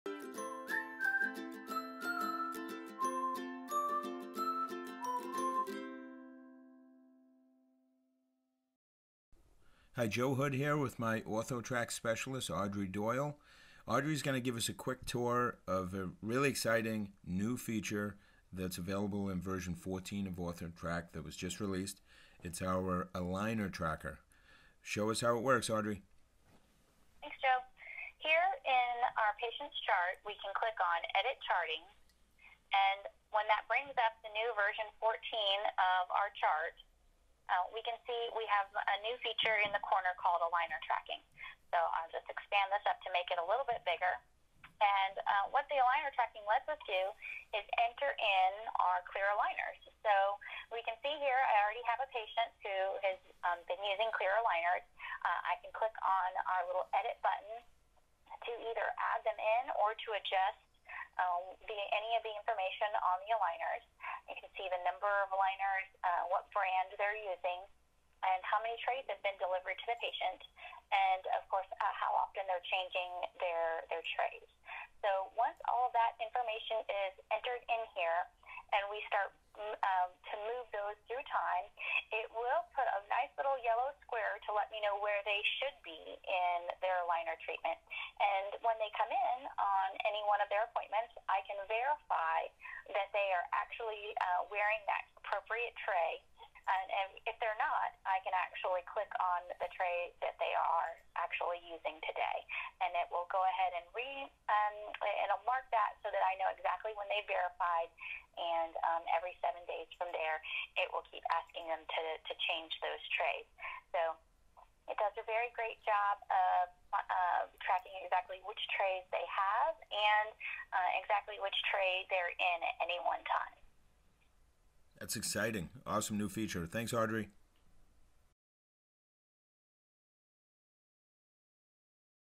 OrthoTrac (V14) - Aligner Tracker
OrthoTrac users, watch this video for a quick tour of the Clear Aligner Tracker feature introduced in Version 14.